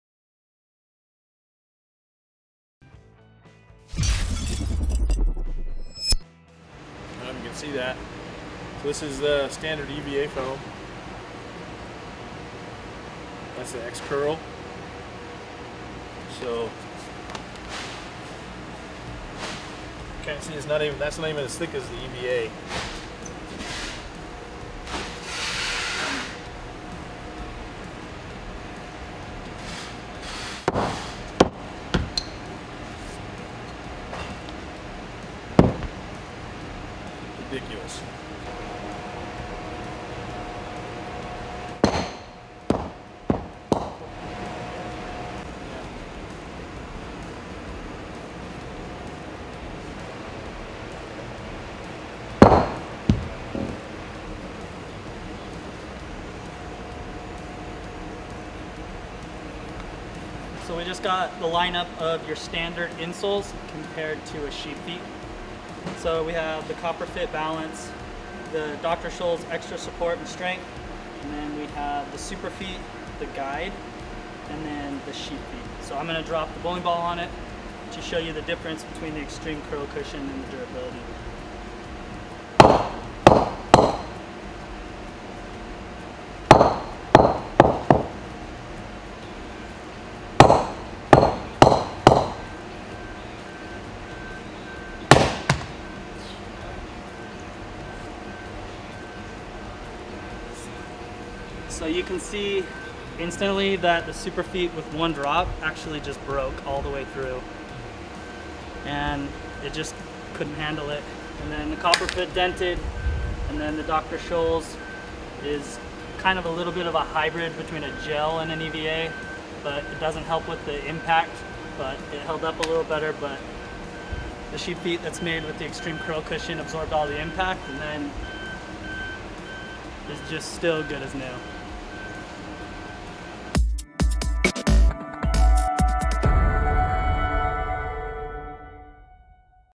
Xtreme Curl Cushion Impact Test
See the difference between standard cushions on the market and the Xtreme Curl Cushion. We drop a bowling ball from 15 feet high to compare the impact absorption.